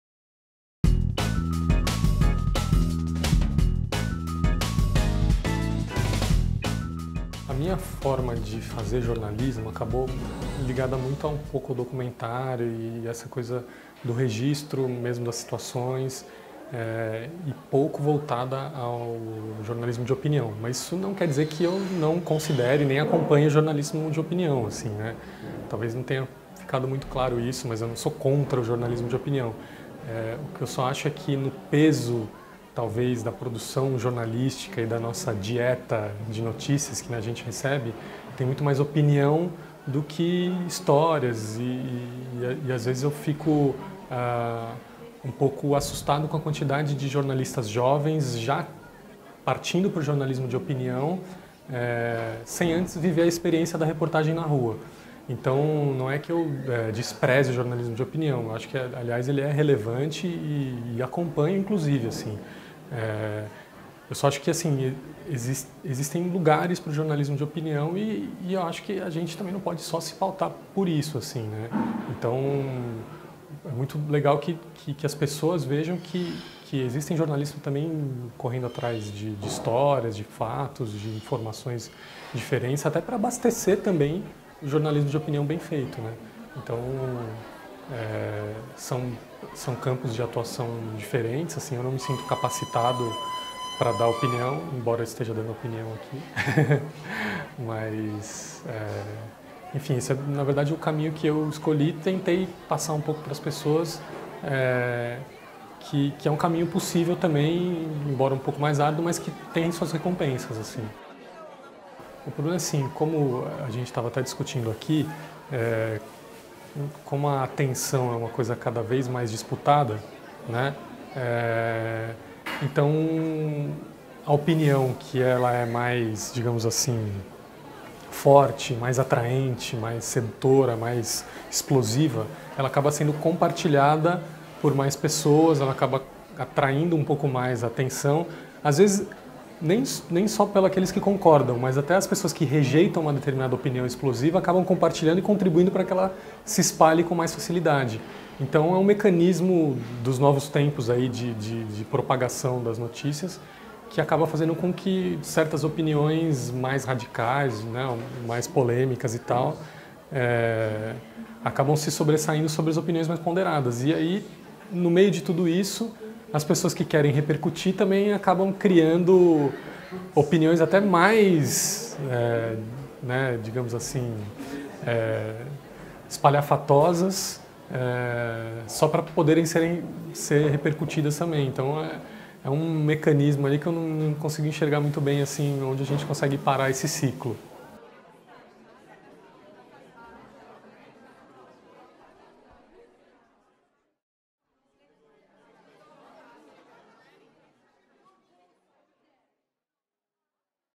Conversa com Caio Cavechini - Jornalismo de Opinião
O jornalista do programa "Profissão Repórter" foi um dos convidados presentes no colóquio Novas Formas de Fazer Jornalismo, ocorrido no dia 8 de maio em Santa Maria - RS.